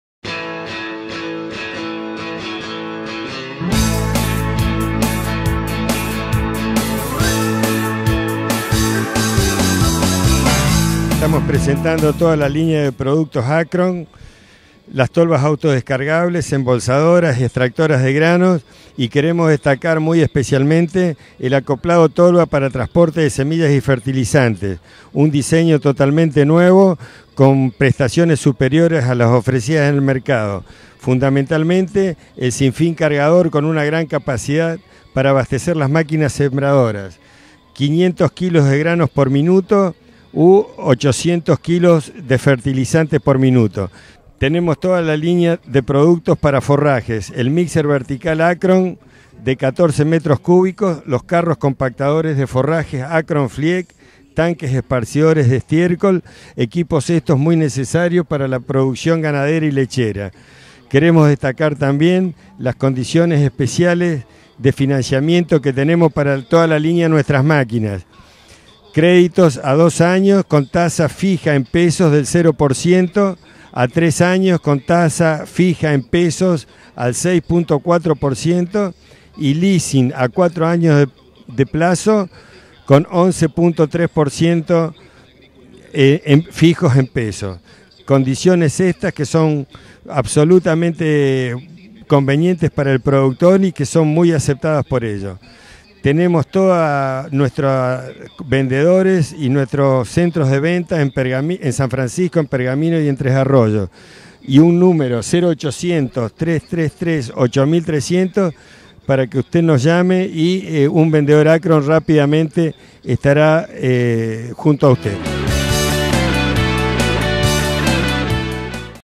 Akron: Nuevo Productos
Alberto Gaviglio, Presidente de Akron, nos presenta la nueva lineas de productos.